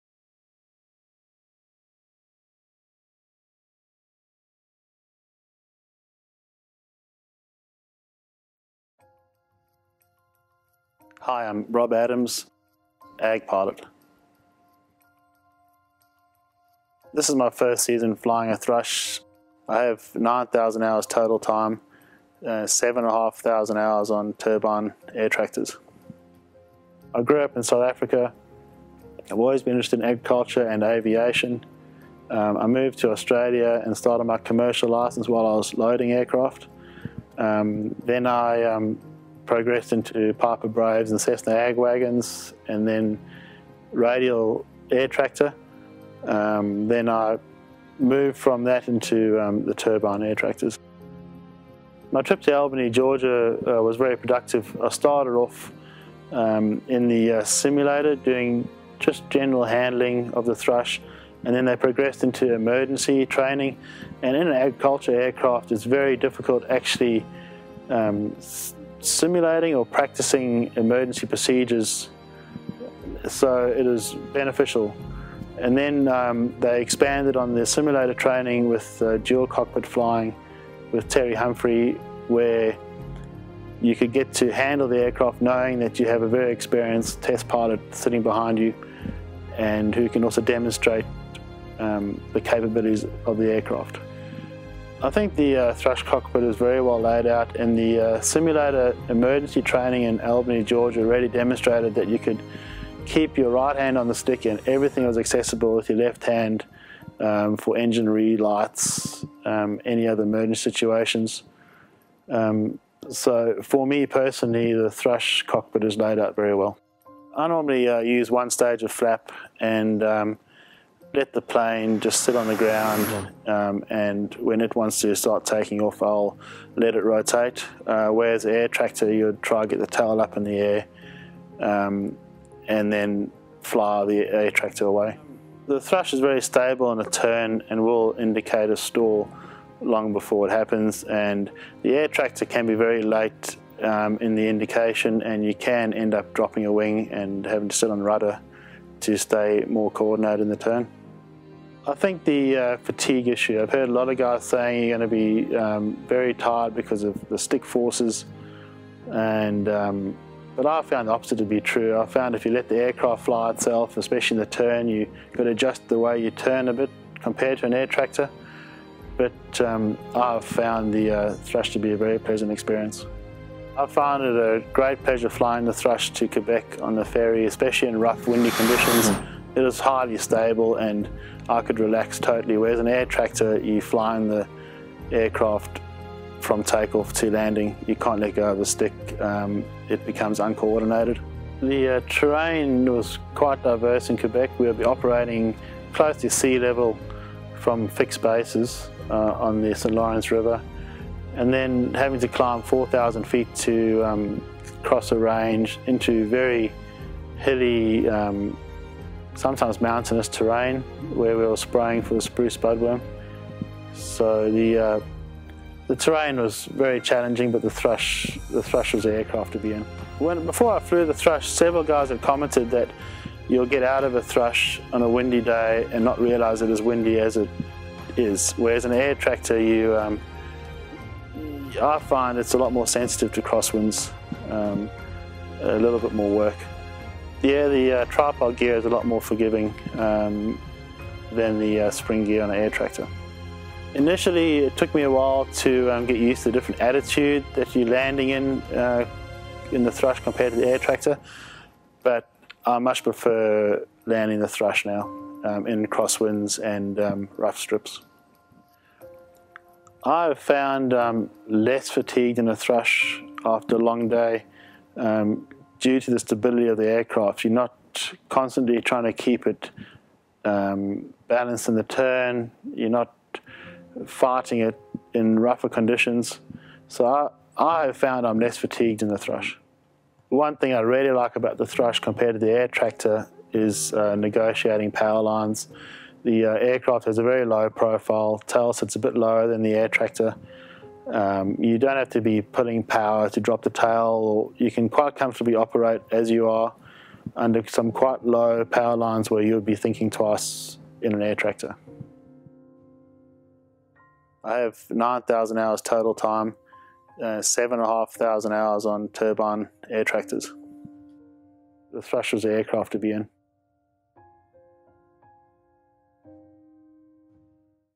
The Aircraft To Be In
Rob Adams, a high time AG pilot, finds himself in a Thrush for the first time in his career. Here he shares his perspective on the Thrush 510 after a full season of doing both forestry and row crop work.

Highlights for Rob include :
Factory Simulator and Aircraft Training
The cockpit
Take-off and landing ease
The stick forces
The flying stability
The gear
Fatigue factors
The aircraft profile